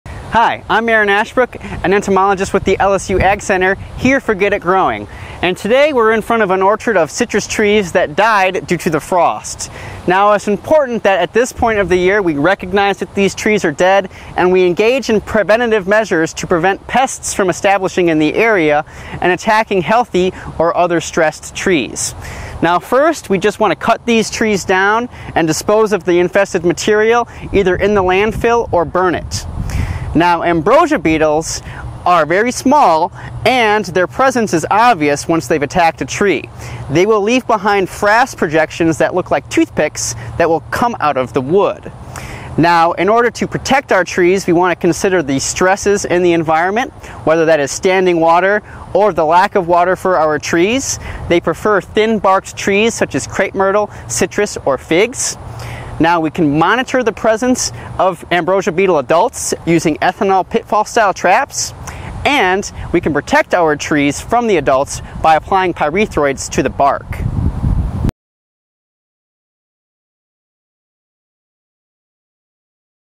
Be on the lookout for the ambrosia beetle | Get It Growing
LSU AgCenter entomologist Aaron Ashbrook provides important information about ambrosia beetles, which have recently been spotted attacking trees in Louisiana. These beetles gravitate toward weakened trees, and infestations can ultimately spread to healthy trees.